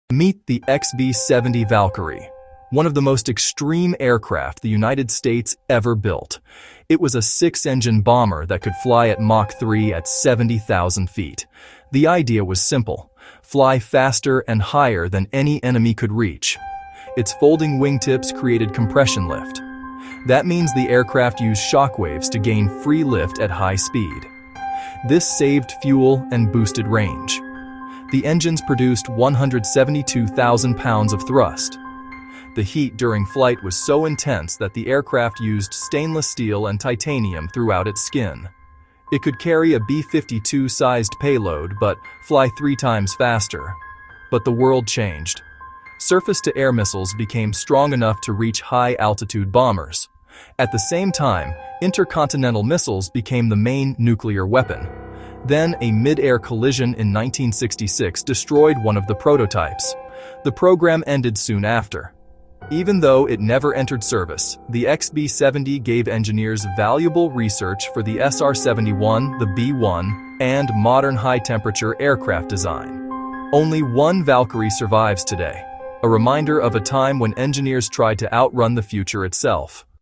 XB-70 Valkyrie — one of the most extreme Cold War aircraft ever built
A short dive into the XB-70 Valkyrie — one of the most extreme Cold War aircraft ever built. Learn how this six-engine, Mach 3, 70,000-ft bomber used folding wingtips for compression lift, stainless steel and titanium skin to survive intense heat, and engines producing 172,000 lbs of thrust. See why it could carry B-52-sized payloads at triple the speed, why missiles and ICBMs ended its role, and how a 1966 midair collision doomed the program — yet its research shaped the SR-71, B-1 and modern high-temp aircraft design. Shot for YouTube Shorts (9:16) — ~1:25 runtime.